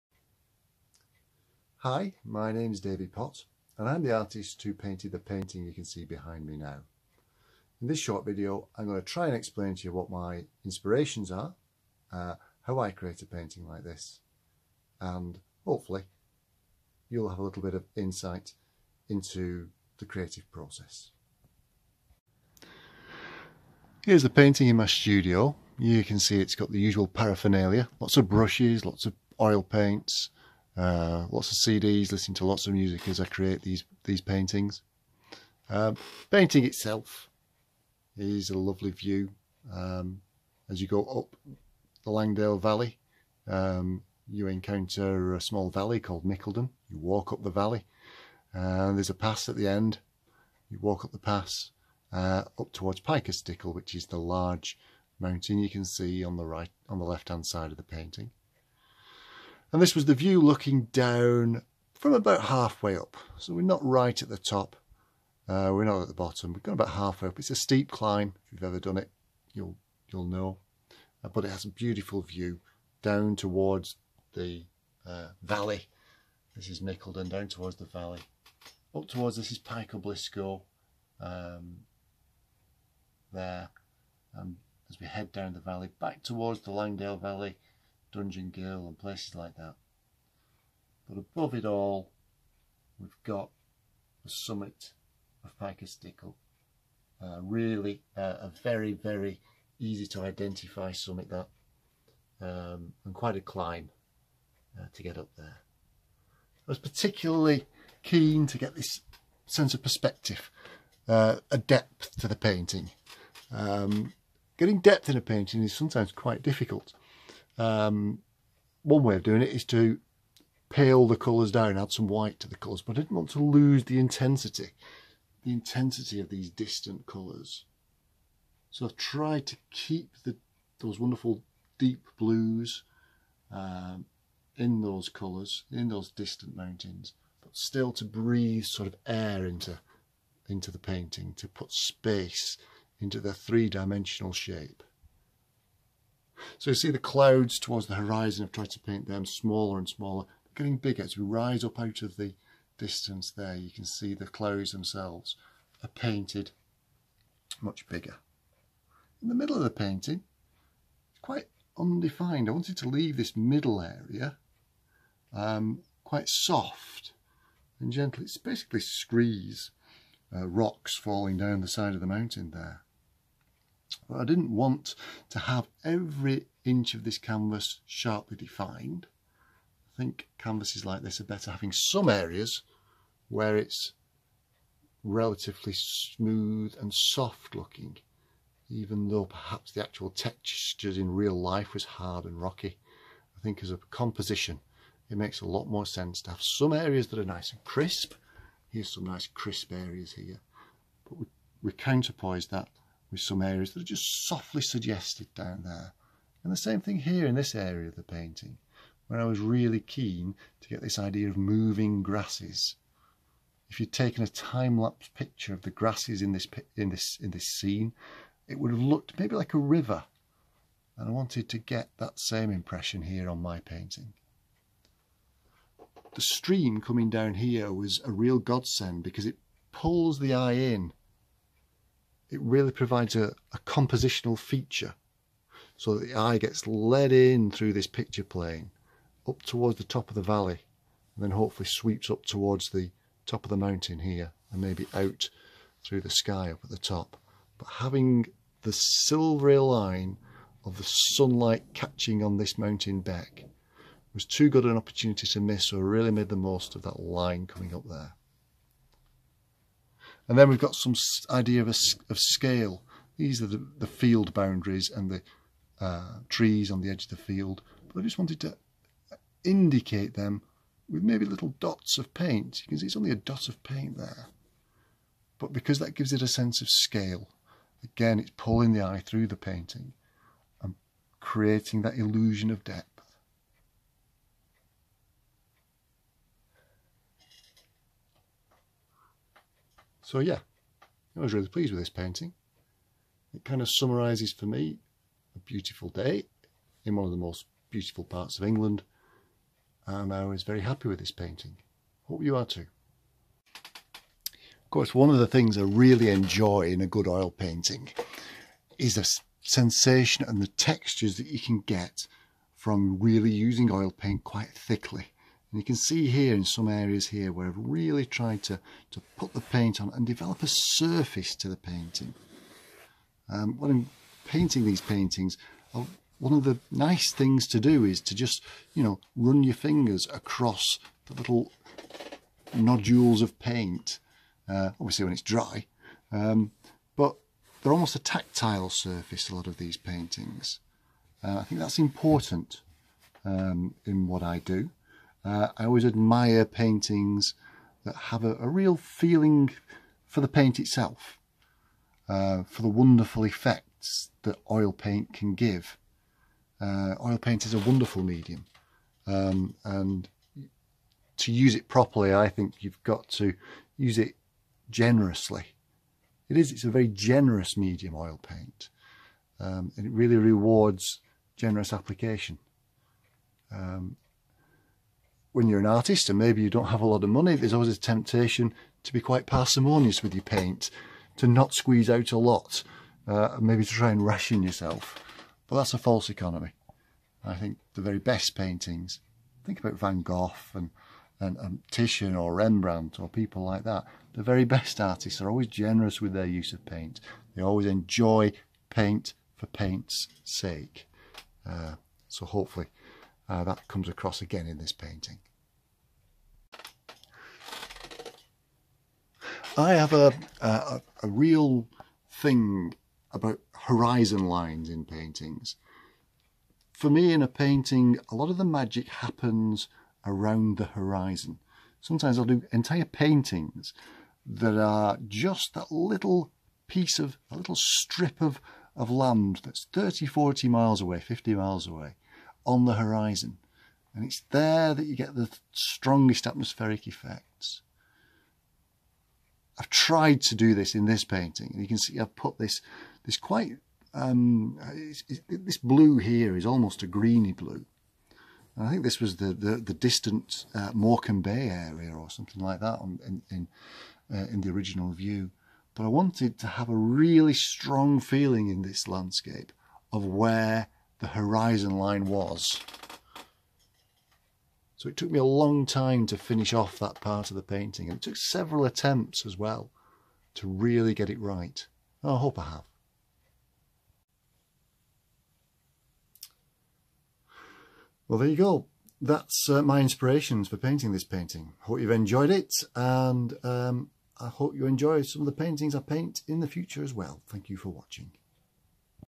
Painting the Lakes - an oil painting of Pike O'Stickle in Langdale, Cumbria
Explaining how I painted one of my paintings. The painting I'm talking about is a landscape painting of Pike of Stickle in the Langdales.